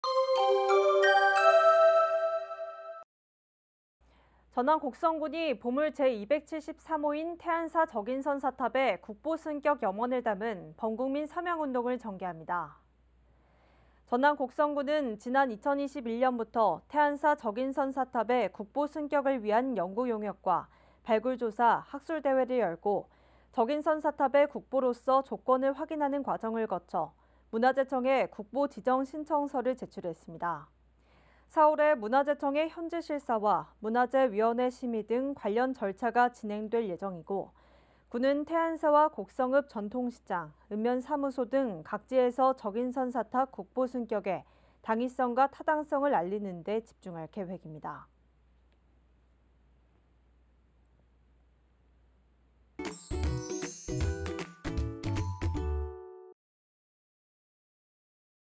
[전남뉴스] 전남 곡성군, '태안사 적인선사탑' 국보승격 기원 서명운동 전개
■CMB뉴스

전남 곡성군이 보물 제273호인 태안사 적인선사탑의 국보 승격 염원을 담은 범군민 서명운동을 전개합니다.

전남 곡성군은 지난 2021년부터 태안사 적인선사탑의 국보 승격을 위한
연구용역과 발굴 조사, 학술대회를 열고 적인선사탑의
국보로써의 조건을 확인하는 과정을 거쳐
문화재청에 국보 지정신청서를 제출했습니다.

4월에 문화재청의 현지실사와 문화재 위원회 심의 등 관련 절차가 진행될 예정이고,
군은 태안사와 곡성읍 전통시장, 읍면 사무소 등
각지에서 적인선사탑 국보 승격의 당위성과 타당성을 알리는 데 집중할 계획입니다.